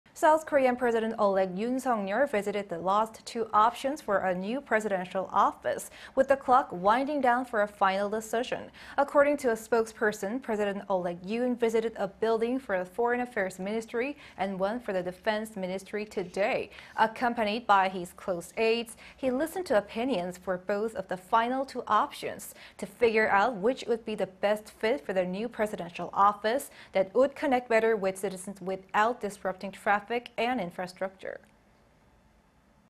President-elect visits the two remaining choices for his new presidential office
윤석열 대통령 집무실 이전 후보지 방문... 직접 의견 들어

South Korean President-elect Yoon Suk-yeol visited the last two options for a new presidential office... with the clock winding down for a final decision.
According to a spokesperson, President-elect Yoon visited a building for the Foreign Affairs Ministry and one for the Defense Ministry... today.
Accompanied by his close aides,... he listened to opinions for both of the final two options... to figure out which would be the best fit for the new presidential office... that would connect better with citizens without disrupting traffic and infrastructure.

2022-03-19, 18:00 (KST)